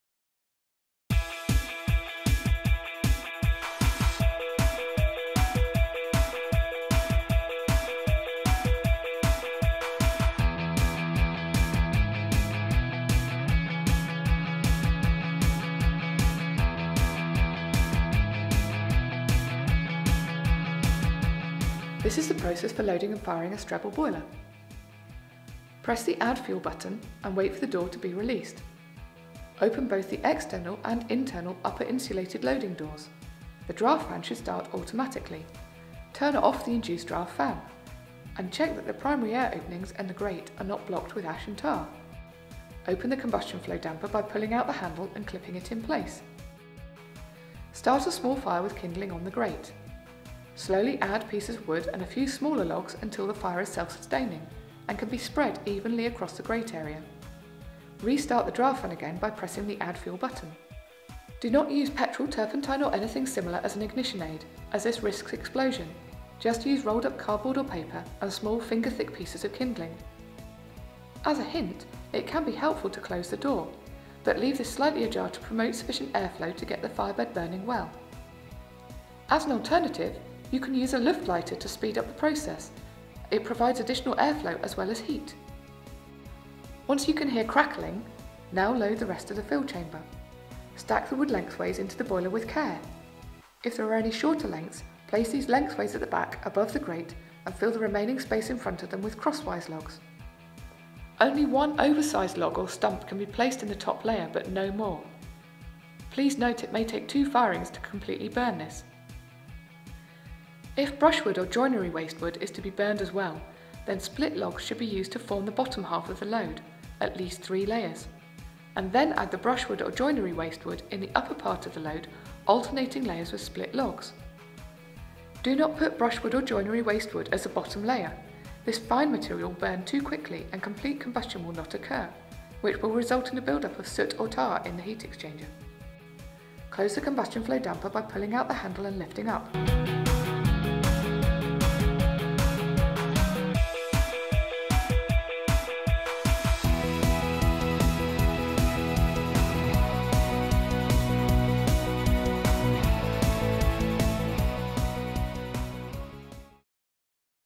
WBS Video 7 - How to light a log boiler
Training video showing how to light a Strebel log boiler - but process applicable to most log boilers